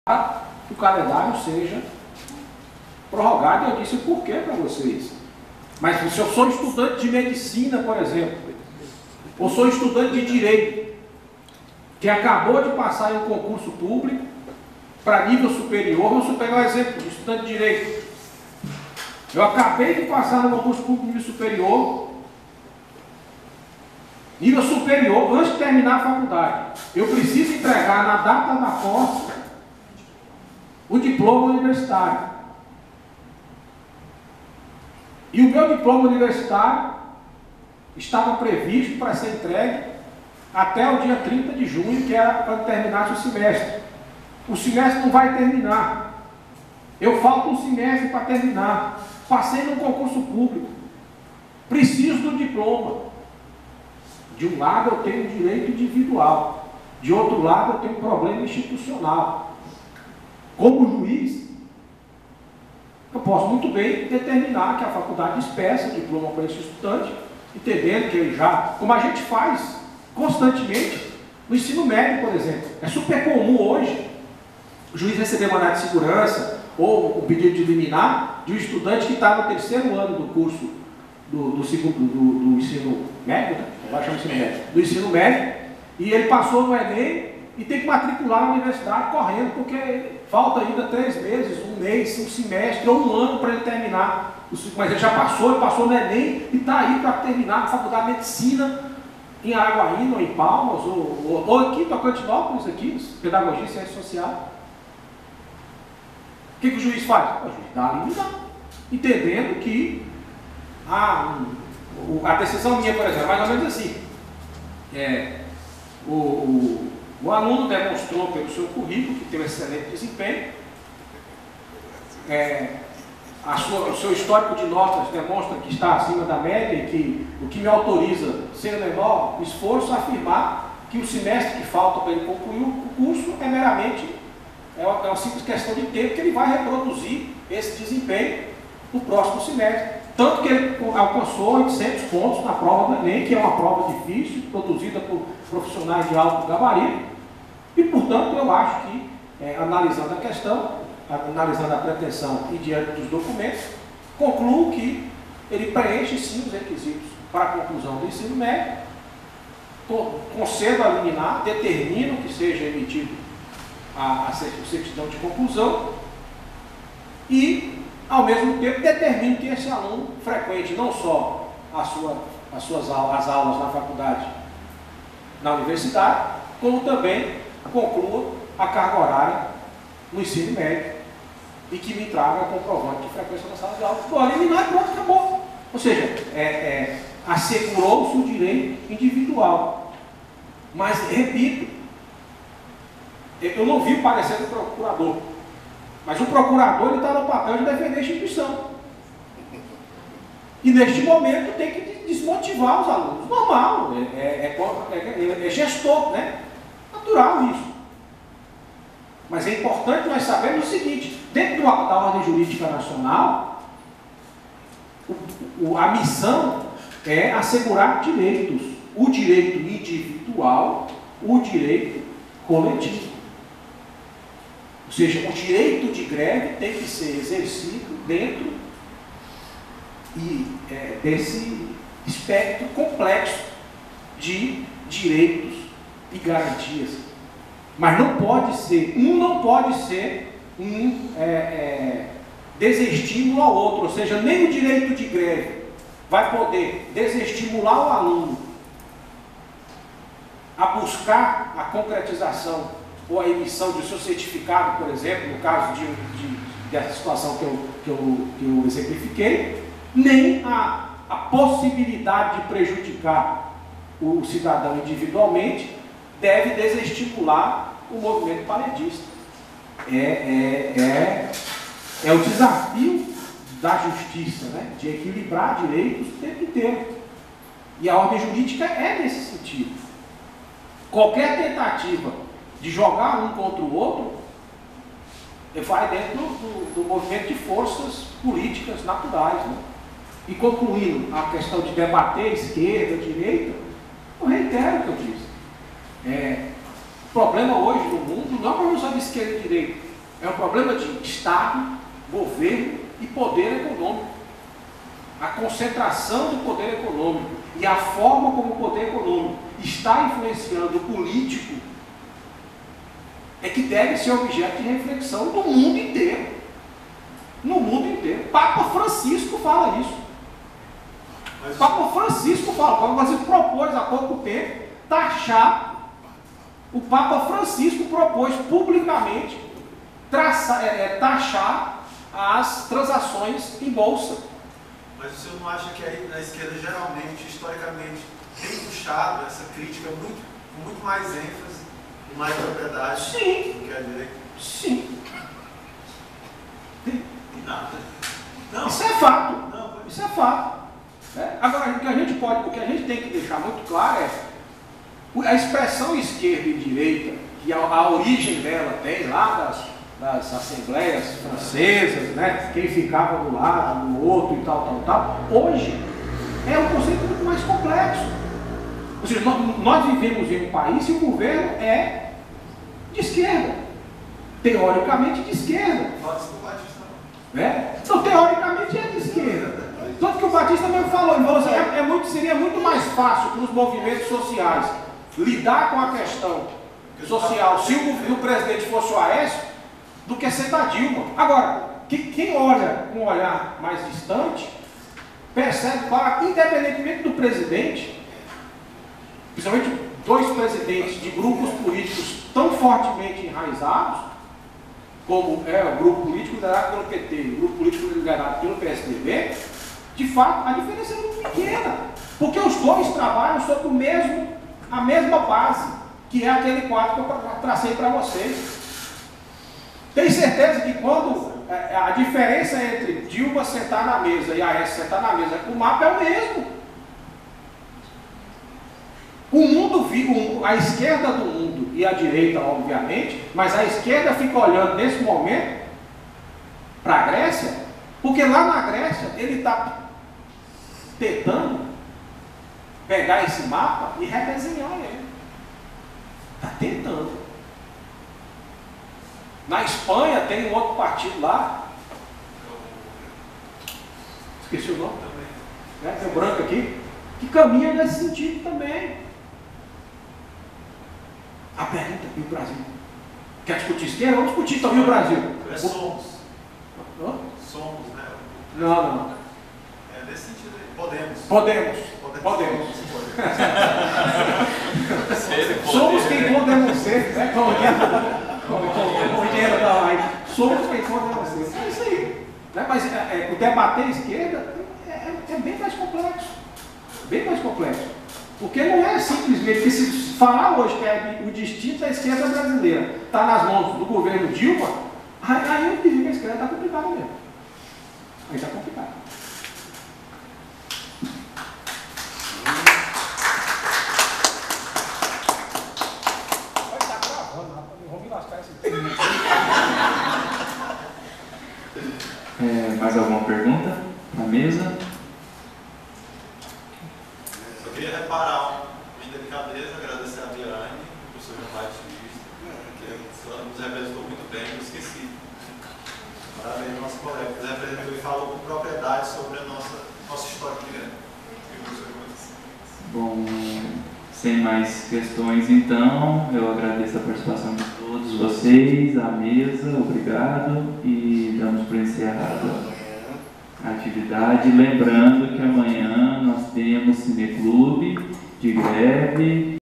Mesa -Redonda Greve:histórico e legislação, no campus de Tocantinópolis (Parte 6)
Este evento aconteceu no dia 18 de junho de 2015, participara, da mesa: Liane Clementino dos Anjos; João Batista de Jesus Felix e  Arióstenes Guimarães Vieira. O moderador foi Mário Borges Netto.